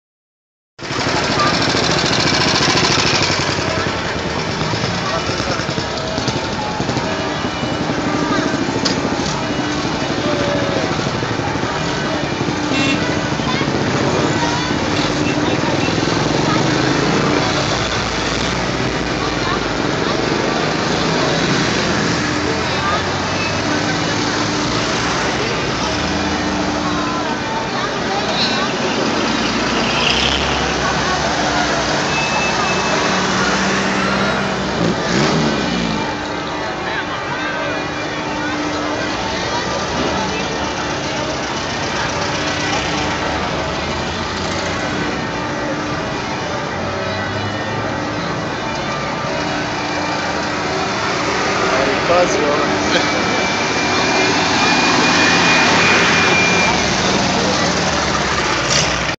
Oldtimer jeeps from second world war
Oldtimer cars (jeeps) from world war II (second world war) in Ljubljana, Slovenia